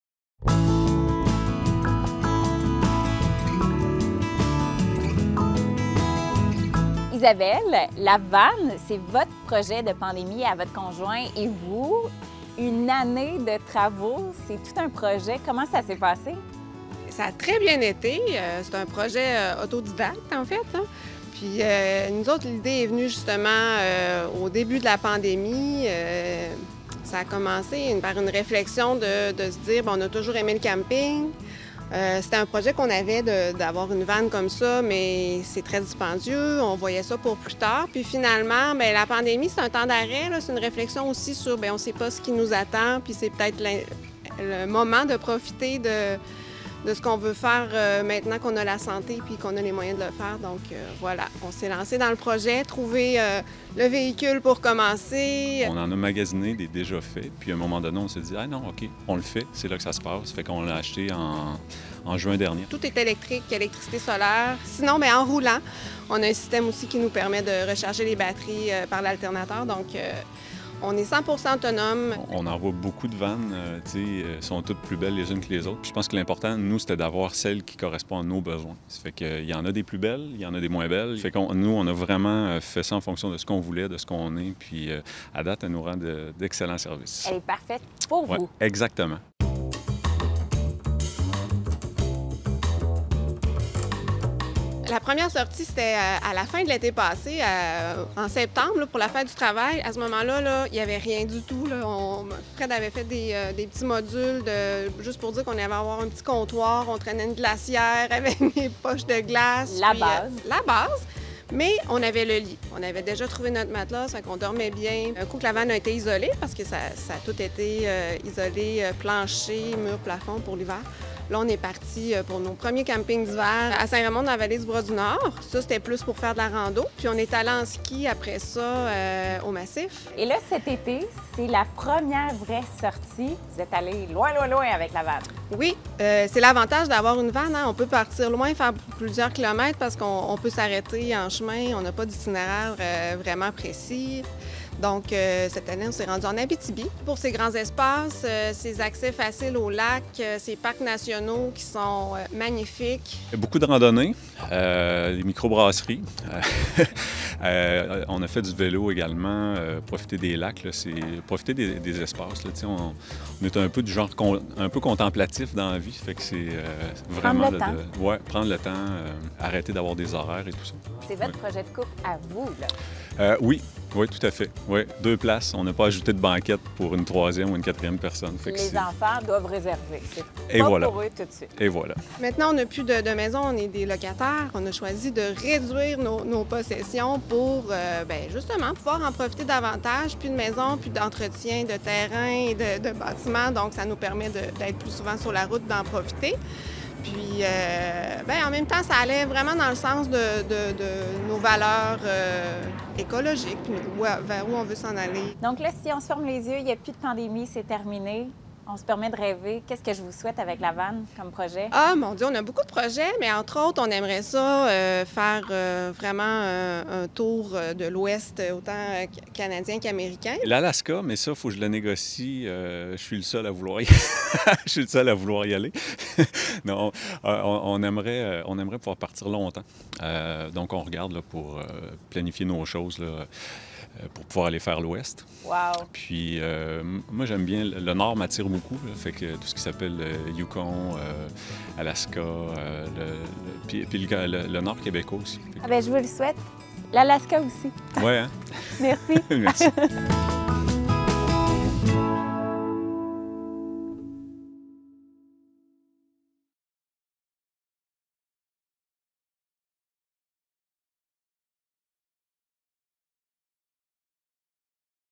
Aménager sa van soi-même : un vieux rêve devenu réalité pour un couple de Québec
Un couple pour qui la pandémie a été le coup d'envoi afin de réaliser un vieux rêve d'aménager une van pour partir à l'aventure.

Suivez ICI Québec sur différentes plateformes